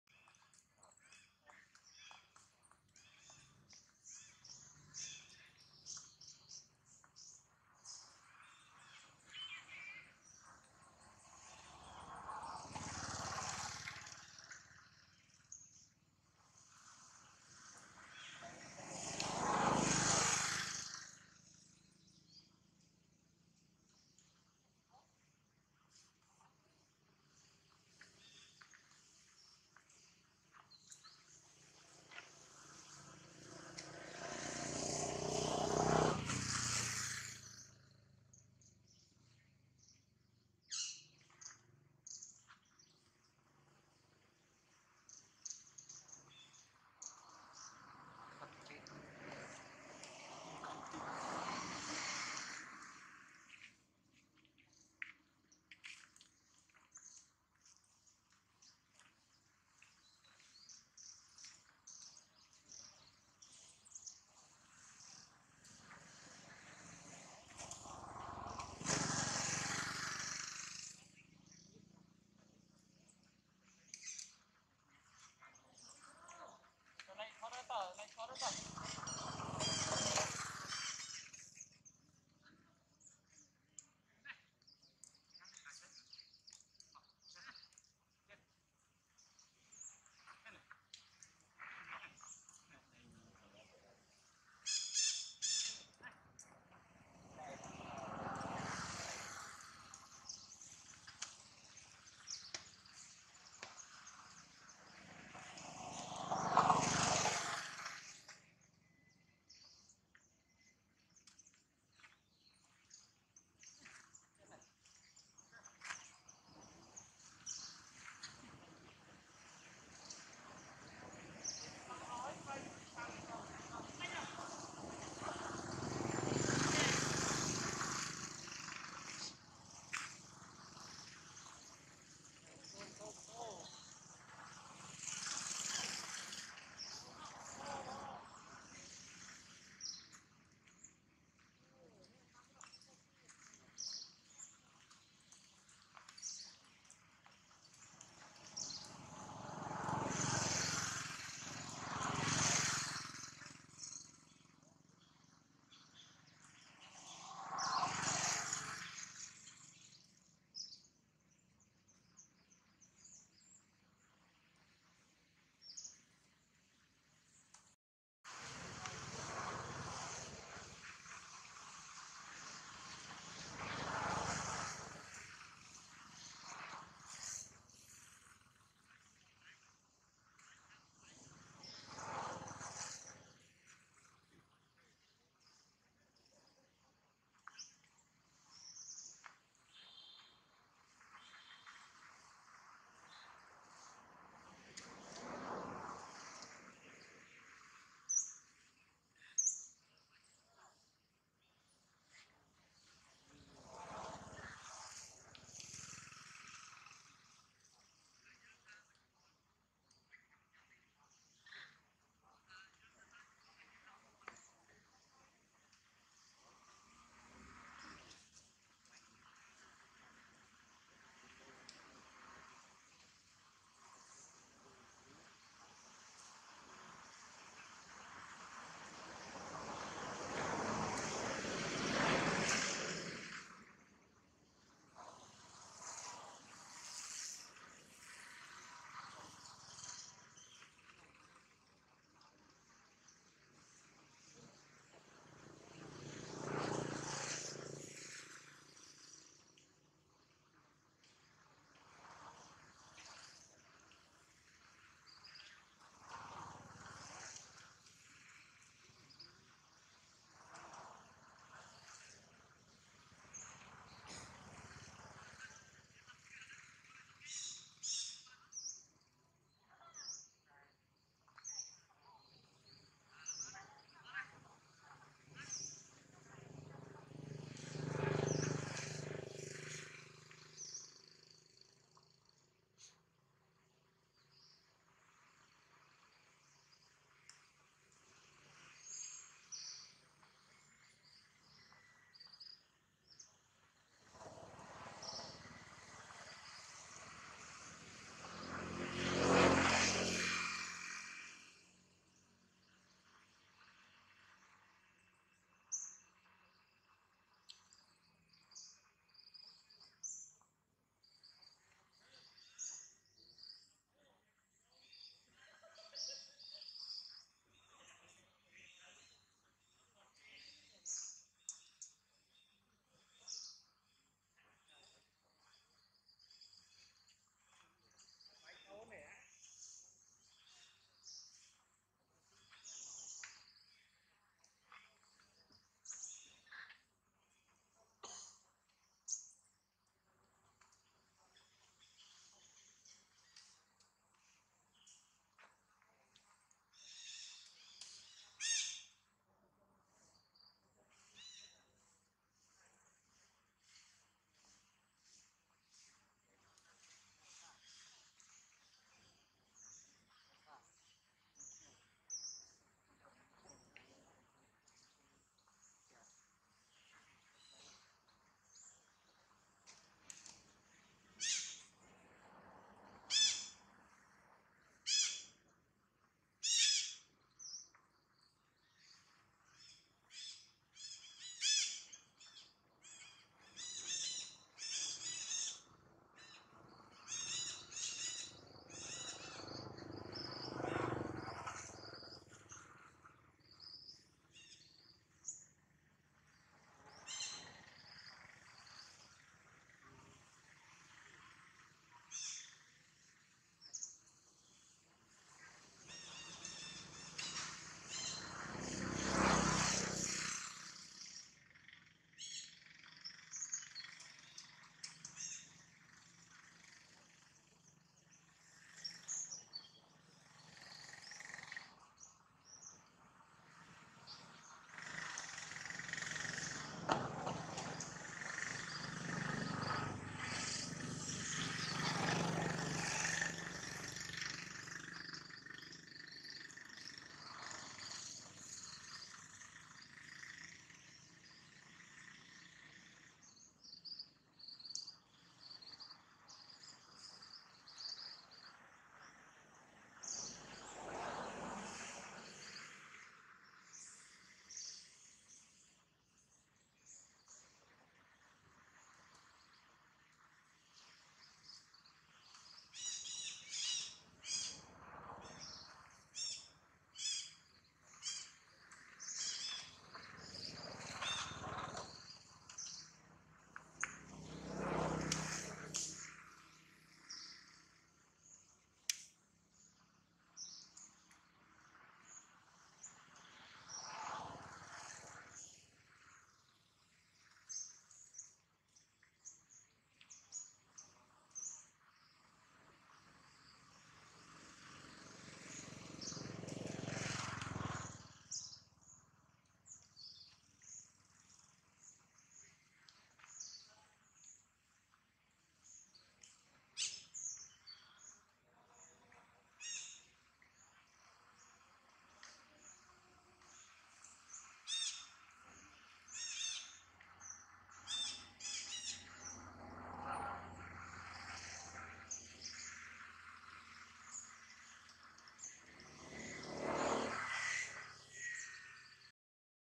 Poor Jane look so tried on green grass alone , Not easy for Jane for walking with the biggest belly
Everyday Monkey